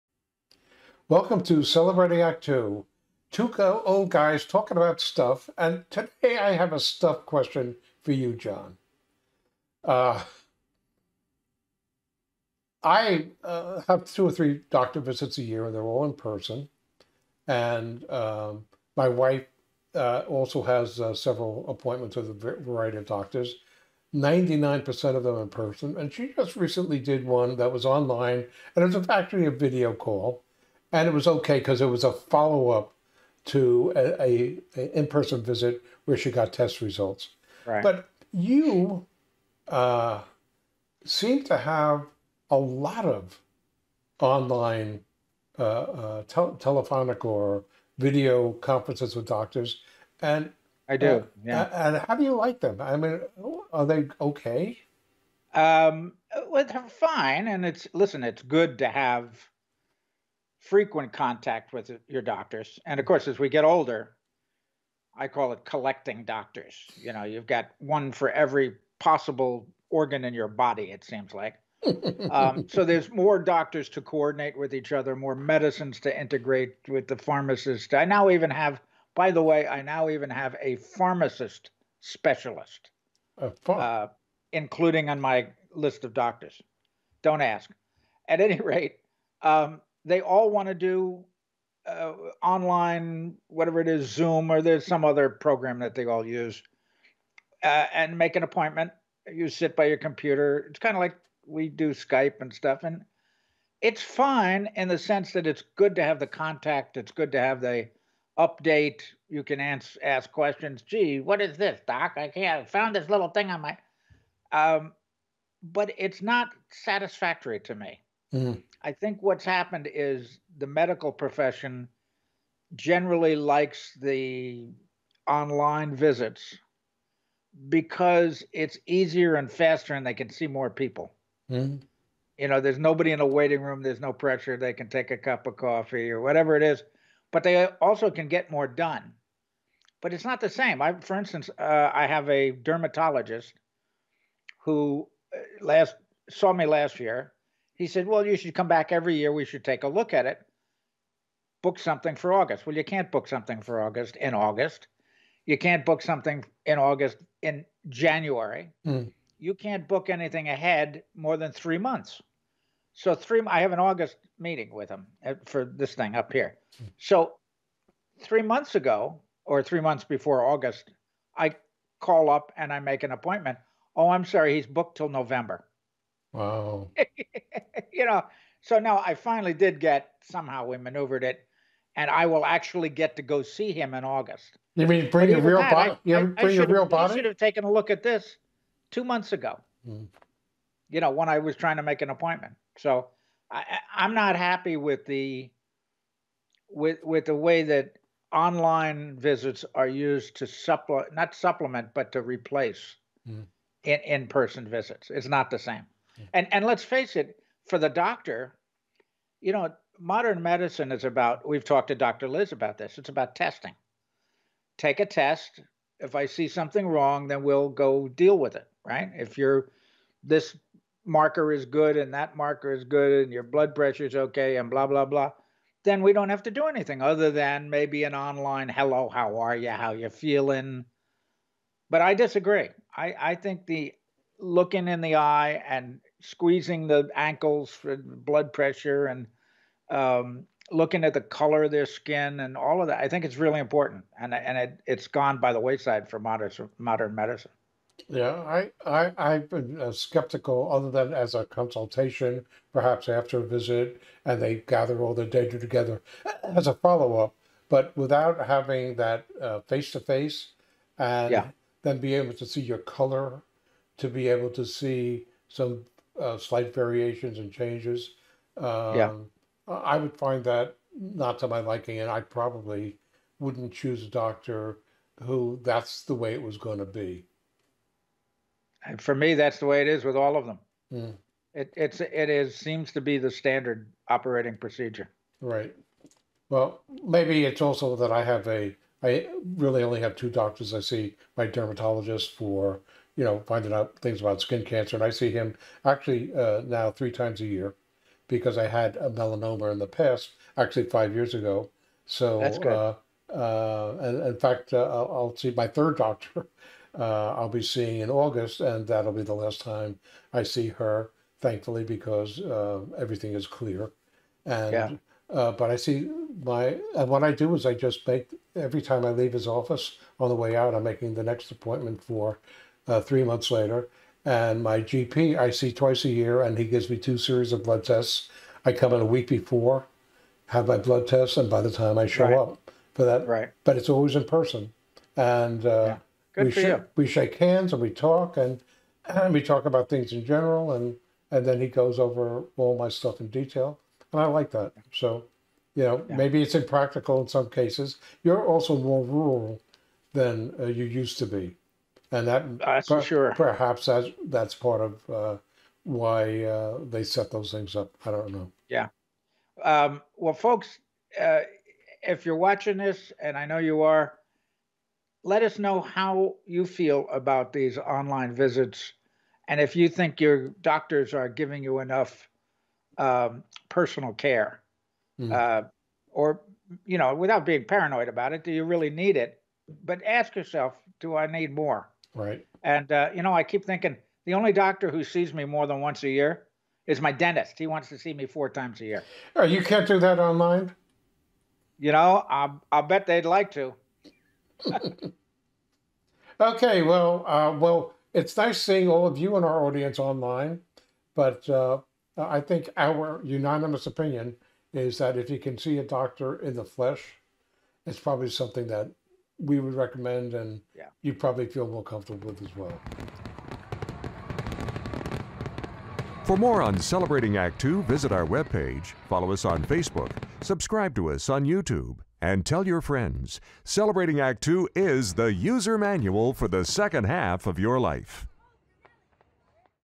Online Doctor Visits
Art & John discuss the phenomena of doctor visits that are increasing done via phone and video conferences.  They are not all that happy with the trend.